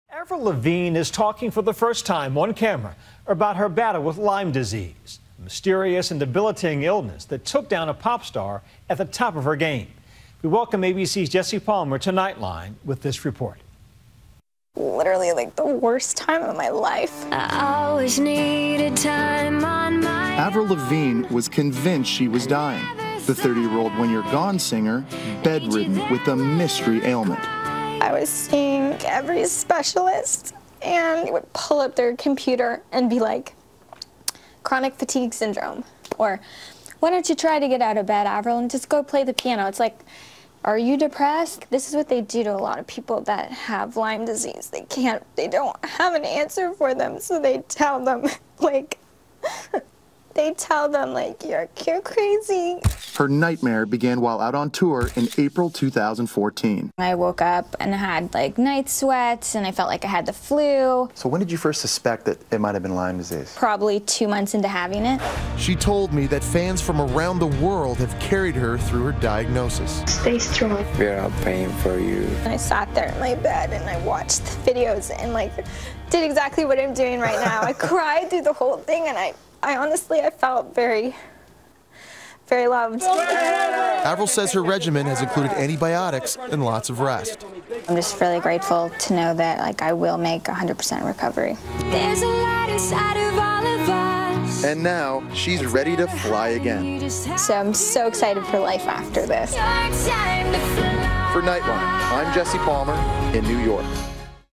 Avril Lavigne Describes Nightmare Experience with Lyme Disease
Pop singer talks about her struggle to get a diagnosis for the illness that put her life and world tour on hold.